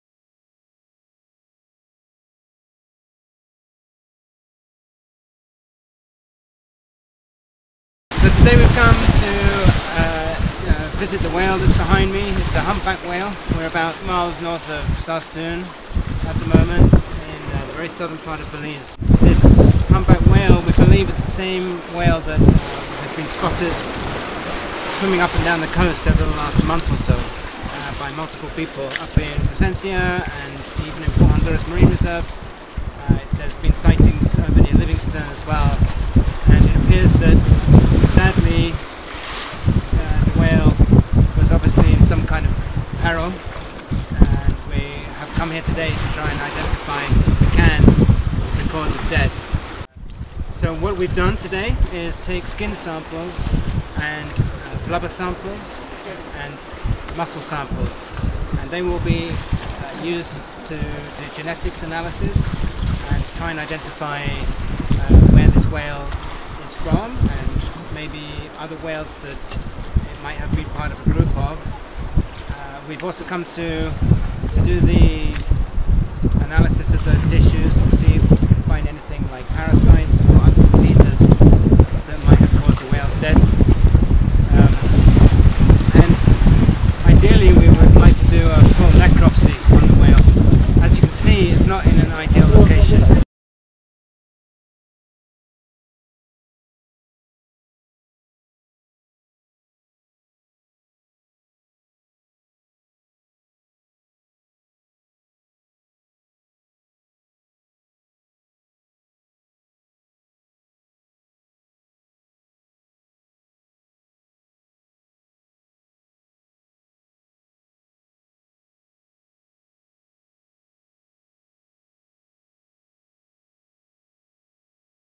Humpback Whale Necropsy, Belize
The Toledo Institute of Development and Environment investigates why the first ever humpback whale to be spotted in Belizean waters has died.

In spite of choppy and murky sea conditions, TIDE succeeded in collecting blubber and skin samples as part of a mission funded by fellow Belizean conservation NGO, Ecomar (26.04.16).

Whilst undertaking the messy job, TIDE’s Science Director, Mr. James Foley and local fisherman, Mr. Hector Saldivar were relentlessly hit by waves that were thick with oil caused by the decomposing whale. Though the midday sun was strong, the visibility of the water was cloudy, making it impossible to spot any potential predators such as sharks or rays coming to feast on the carcass.

In the end, the conditions and a lack of suitable tools meant a full necropsy could not be performed.  Finding out its cause of death presents a rare opportunity for insight to the whale and conservation communities, and could go a long way in understanding why this humpback had been traveling up and down the coastal line of Belize and Guatemala since it was first spotted in late January 2016.